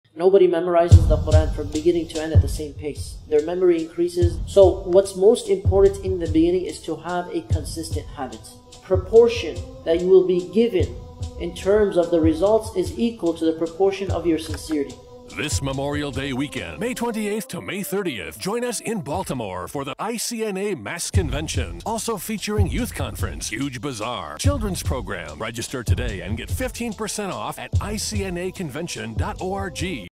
Sh. Suleiman Hani | ICNA-MAS Convention | May 28-30 | Baltimore, MD
The ICNA-MAS annual convention is one of the largest gatherings of American Muslims in the U.S. Over 22,000 Muslims attended the convention in Baltimore, MD. Alhamdulillah.  It featured many top scholars and activists, in addition to many special events like Meals Distribution in Downtown, Mercy Bags distribution to senior homes and Street dawah near the White House.